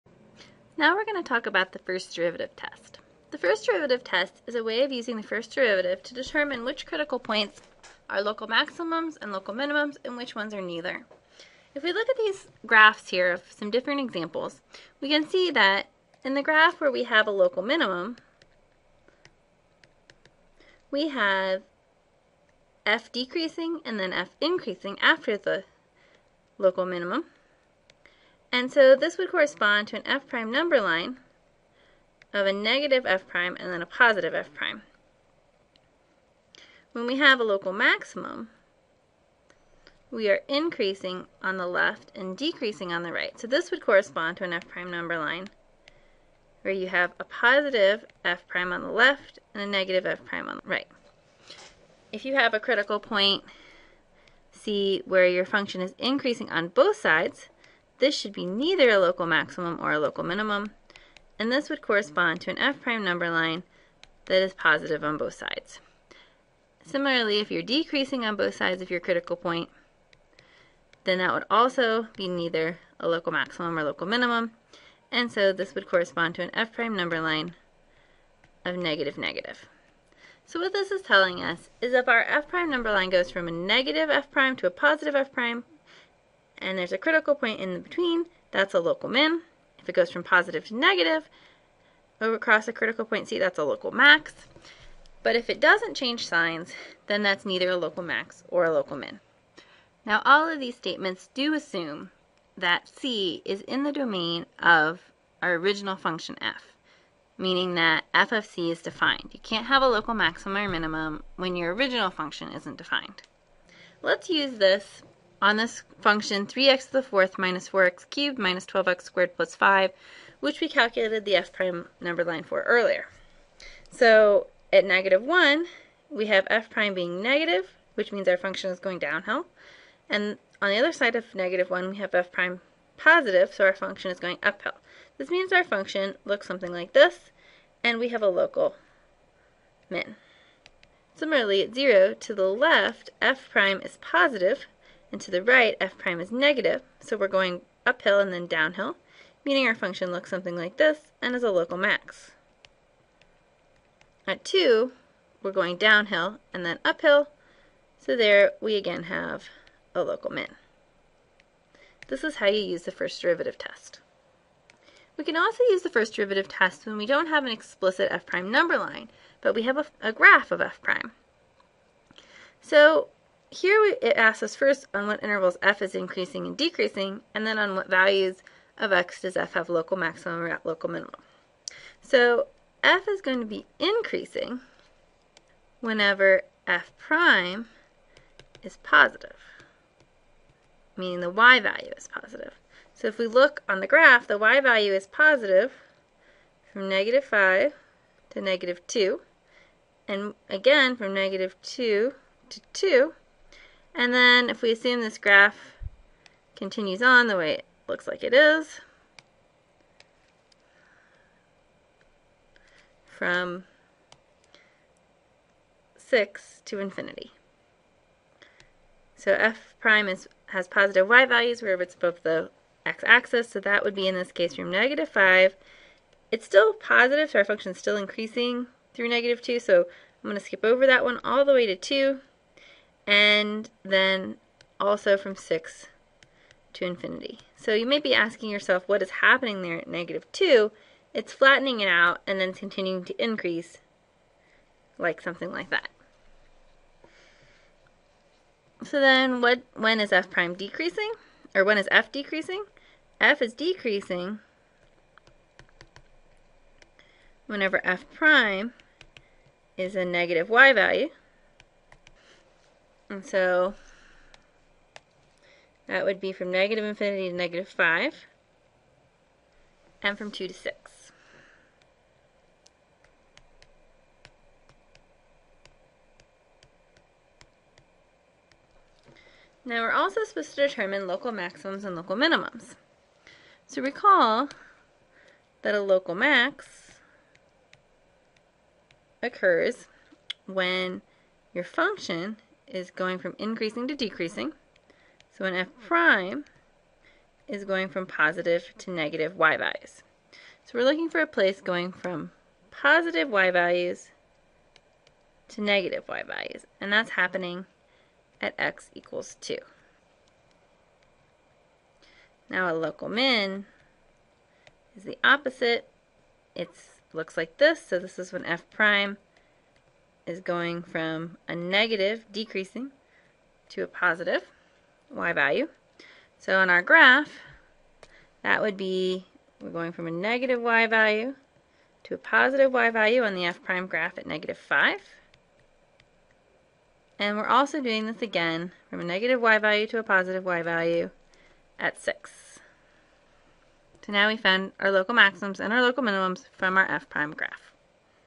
First Derivative Test
This video explains the first derivative test and shows how to use it to determine whether critical points of a function are local maximums, local minimums, or neither.